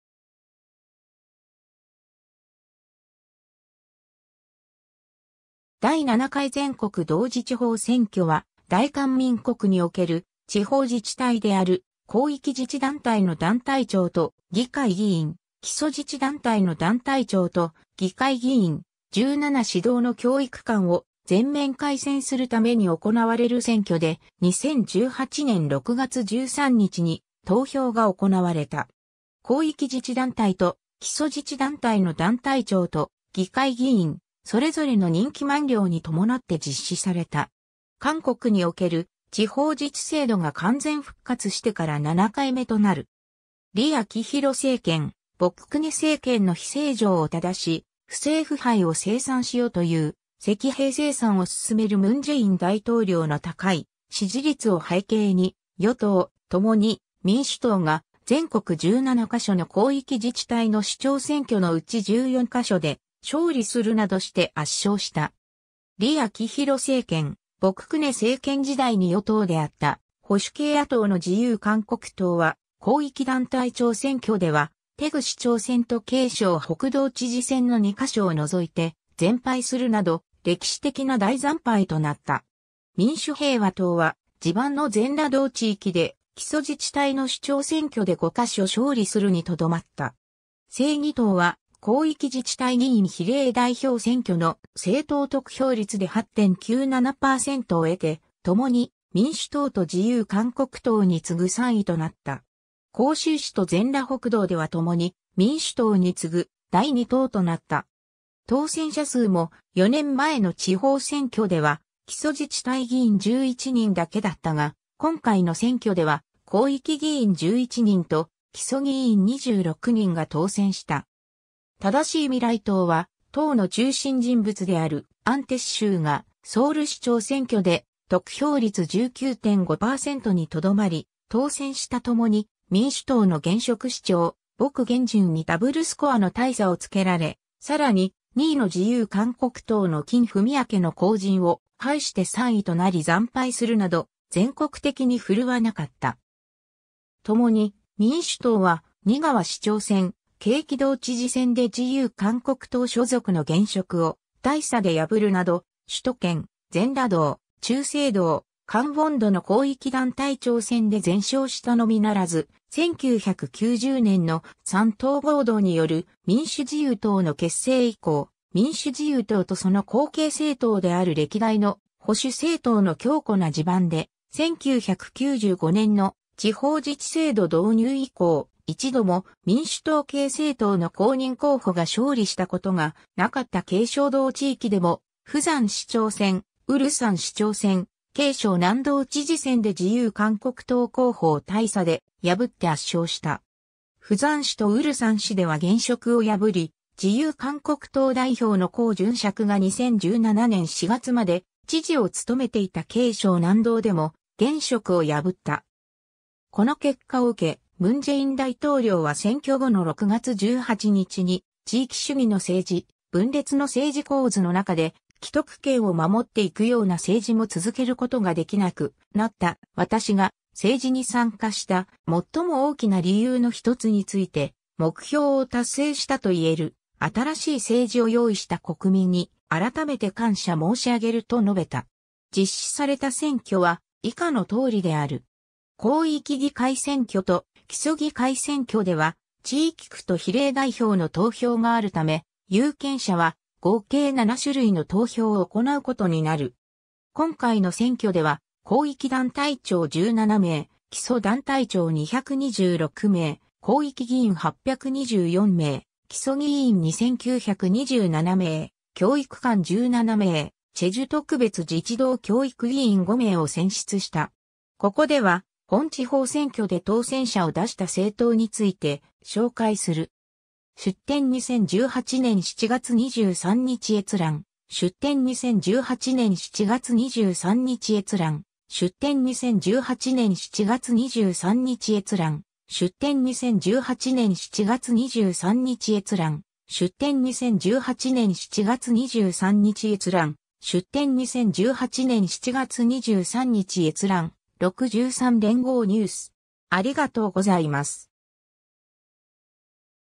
第7回全国同時地方選挙 (韓国)
第7回全国同時地方選挙（だい7かいぜんこくどうじちほうせんきょ）は、大韓民国における地方自治体である広域自治団体（特別市・広域市、道）の団体長（市長・知事）と議会議員、基礎自治団体（一般市、郡、特別市・広域市の区）の団体長（市長・郡守・区長（直訳は区庁長））と議会議員、17市道の教育監（日本の教育長に相当）を全面改選するために行われる選挙で、2018年6月13日に投票が行われた。
 広域自治団体と基礎自治団体の団体長と議会議員、それぞれの任期満了に伴って実施された。
韓国における地方自治制度が完全復活してから7回目となる。
 李明博政権・朴槿恵政権の非正常を正し、不正腐敗を清算しようという「積弊清算」を進める文在寅大統領の高い支持率を背景に、与党・共に民主党が全国17か所の広域自治体の首長（広域団体長）選挙のうち14か所で勝利するなどして圧勝した。
 李明博政権・朴槿恵政権時代に与党であった保守系野党の自由韓国党は広域団体長選挙では大邱市長選と慶尚北道知事選の2か所を除いて全敗するなど、歴史的な大惨敗となった。
 民主平和党は、地盤の全羅道地域で基礎自治体の首長選挙で5か所勝利するにとどまった。
 正義党は広域自治体議員比例代表選挙の政党得票率で8.97%を得て、共に民主党と自由韓国党に次ぐ3位となった。
光州市と全羅北道では共に民主党に次ぐ第二党となった。
当選者数も、4年前の地方選挙では基礎自治体議員11人だけだったが、今回の選挙では広域議員11人と基礎議員26人が当選した。
 正しい未来党は党の中心人物である安哲秀がソウル市長選挙で得票率19.5%にとどまり、当選した共に民主党の現職市長・朴元淳（得票率52.7%）にダブルスコアの大差をつけられ、さらに、2位の自由韓国党の金文洙（得票率23.3％）の後塵を拝して3位となり惨敗するなど、全国的に振るわなかった。
 共に民主党は仁川市長選、京畿道知事選で自由韓国党所属の現職を大差で破るなど、首都圏、全羅道、忠清道、江原道の広域団体長選で全勝したのみならず、1990年の三党合同による民主自由党の結成以降、民主自由党とその後継政党である歴代の保守政党（民主自由党→新韓国党→ハンナラ党→セヌリ党→自由韓国党）の強固な地盤で、1995年の地方自治制度導入以降、一度も民主党系政党の公認候補が勝利したことがなかった慶尚道地域でも、釜山市長選、蔚山市長選、慶尚南道知事選で自由韓国党候補を大差で破って圧勝した。
釜山市と蔚山市では現職を破り、自由韓国党代表の洪準杓が2017年4月まで知事を務めていた慶尚南道でも元職を破った。
 この結果を受け、文在寅大統領は選挙後の6月18日に「地域主義の政治、分裂の政治構図の中で既得権を守っていくような政治も続けることができなくなった」「私が政治に参加した最も大きな理由の一つについて目標を達成したと言える」「新しい政治を用意した国民に改めて感謝申し上げる」と述べた。
 実施された選挙は以下のとおりである。
広域議会選挙と基礎議会選挙では地域区と比例代表の投票（候補者投票と政党投票）があるため、有権者は合計七種類（世宗特別自治市は四種類、済州特別自治道は五種類）の投票を行うことになる。
 今回の選挙では広域団体長17名、基礎団体長226名、広域議員824名、基礎議員2927名、教育監17名、済州特別自治道教育議員5名を選出した。
 ここでは本地方選挙で当選者を出した政党について紹介する。